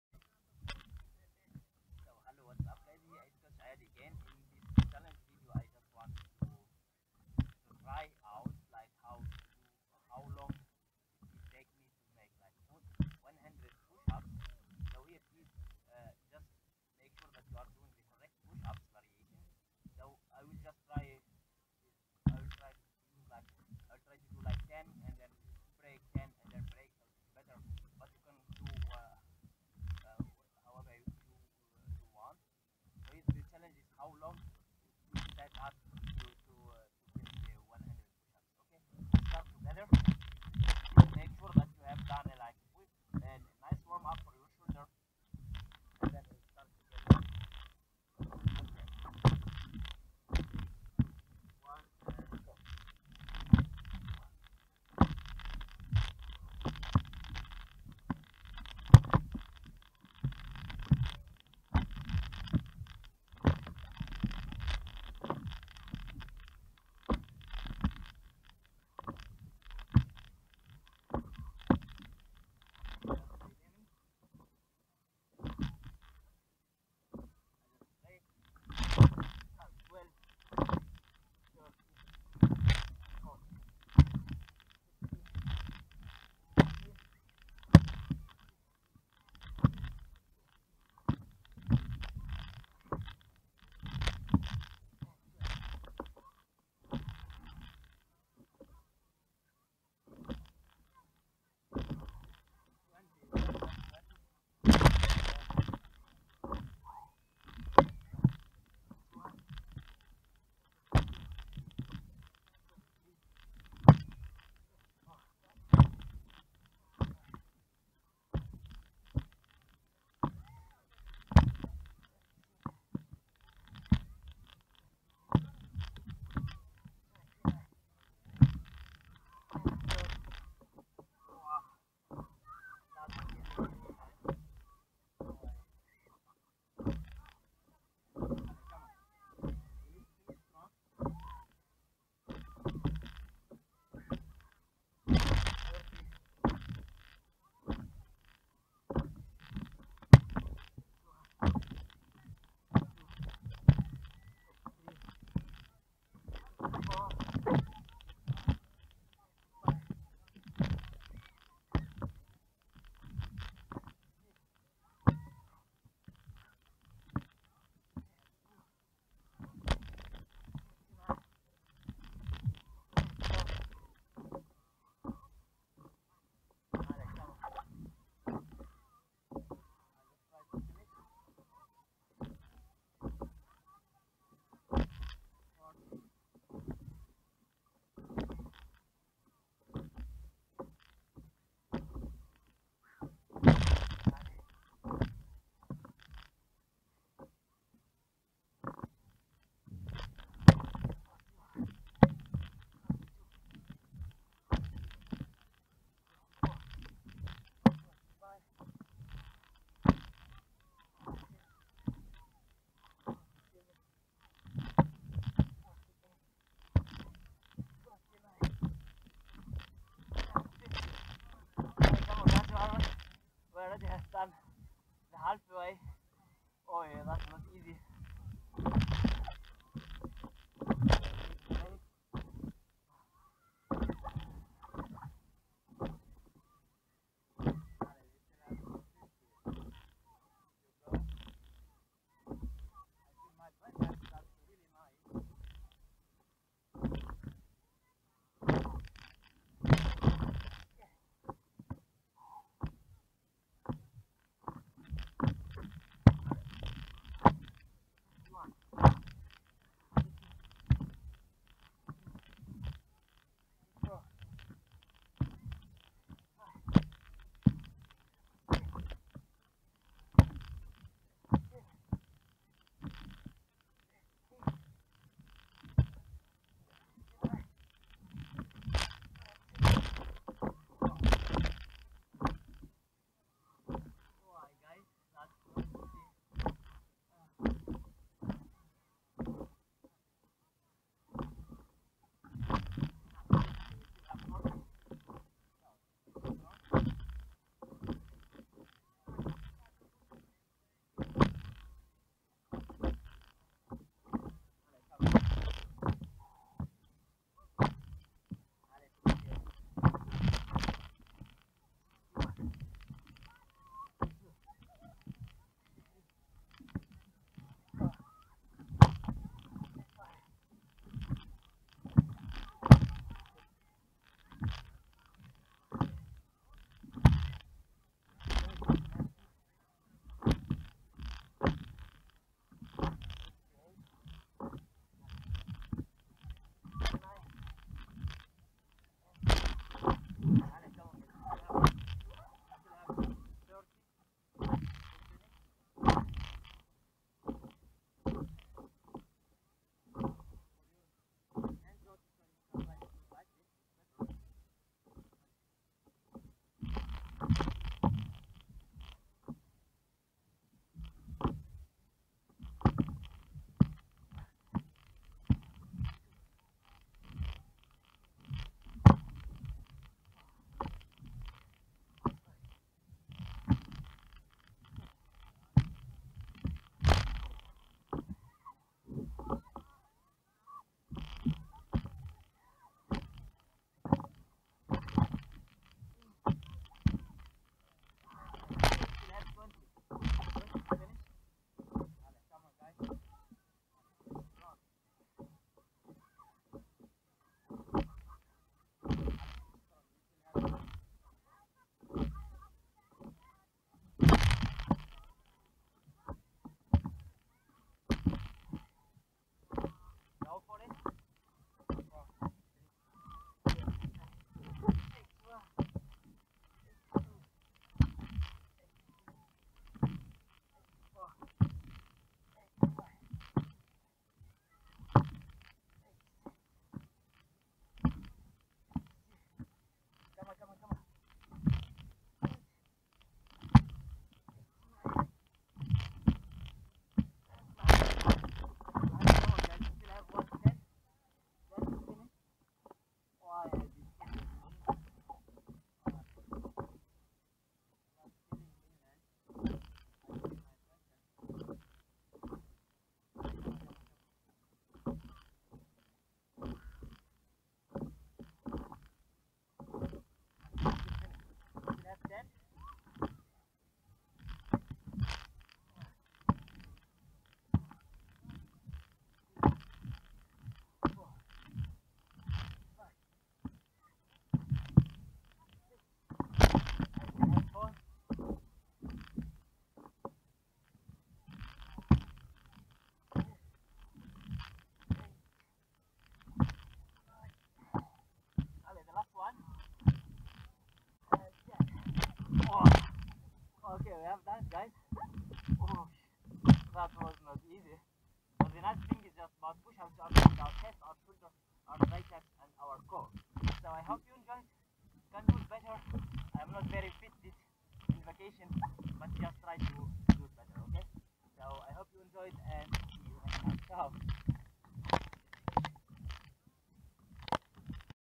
100 Pushups Challenge | Do It Better
Here we make just a challenge: how long does it take to finish 100 pushups.